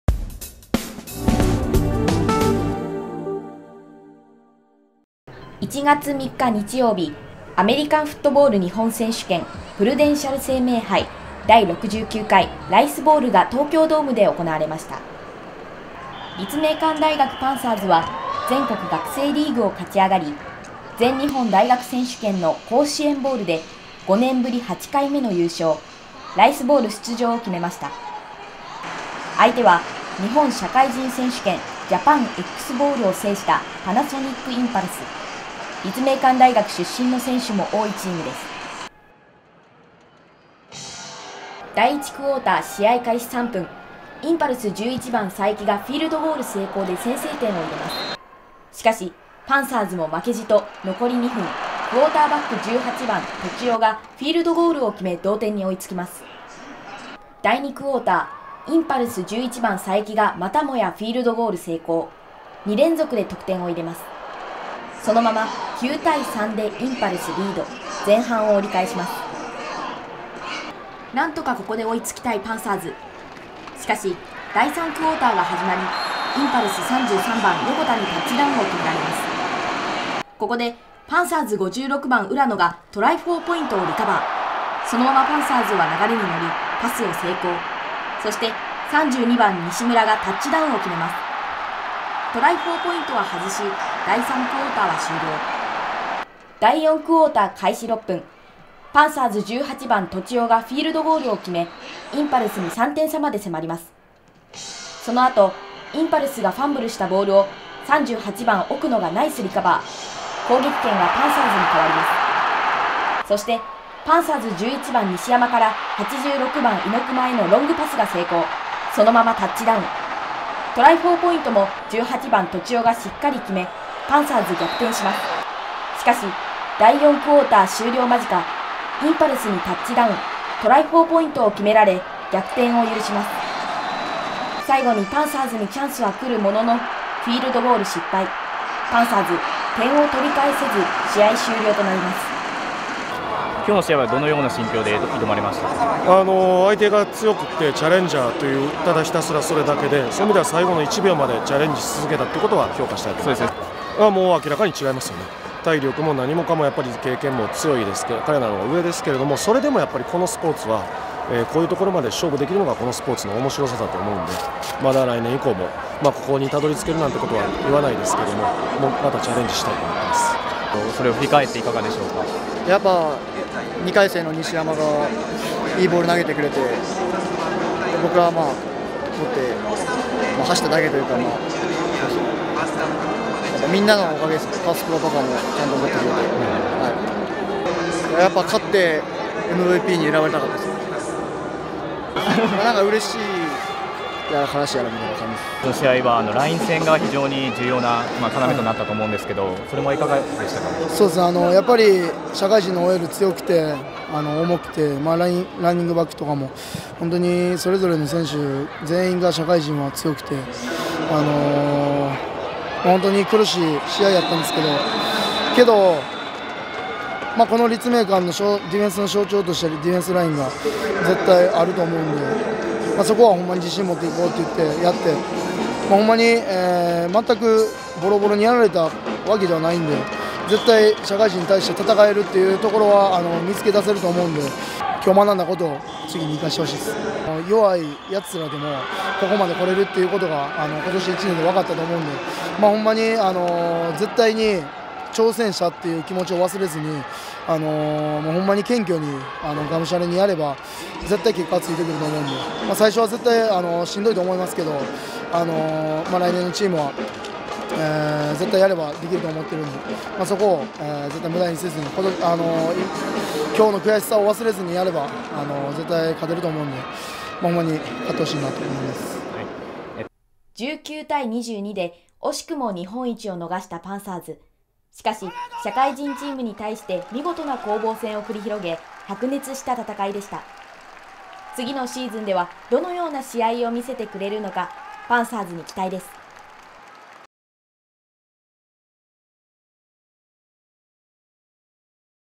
アメリカンフットボール日本選手権第69回ライスボウル 立命館大学パンサーズVSパナソニックインパルス 【2016年1月3日取材】
1月3日㈰、アメリカンフットボール日本選手権プルデンシャル生命杯第69回ライスボウルが東京ドームで行われました。5年ぶりの出場を決めた立命館大学パンサーズは、立命館大学出身の選手も多い社会人チームパナソニックインパルスとの白熱した戦いを繰り広げました。